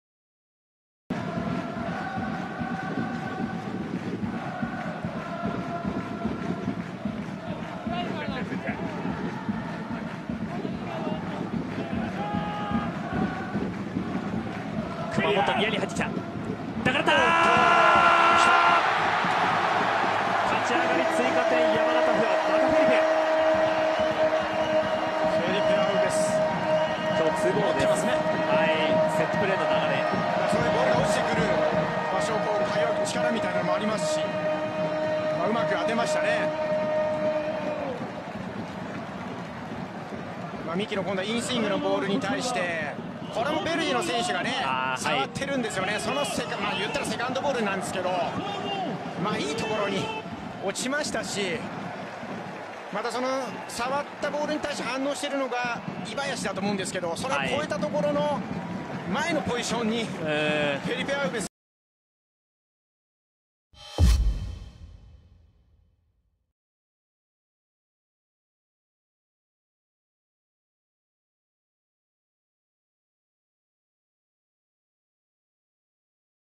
【公式】ゴール動画：フェリペ アウベス（山形）48分 モンテディオ山形vs東京ヴェルディ 明治安田生命Ｊ２リーグ 第28節 2018/8/11
左CKからのボールが相手に当たってファーサイドに流れ、フェリペ アウベス（山形）が押し込んで山形２点目！明治安田Ｊ２のゴール動画を公開中！
2018年8月11日（土）に行われた明治安田生命Ｊ２リーグ 第28節 山形vs東京Ｖのゴール動画です。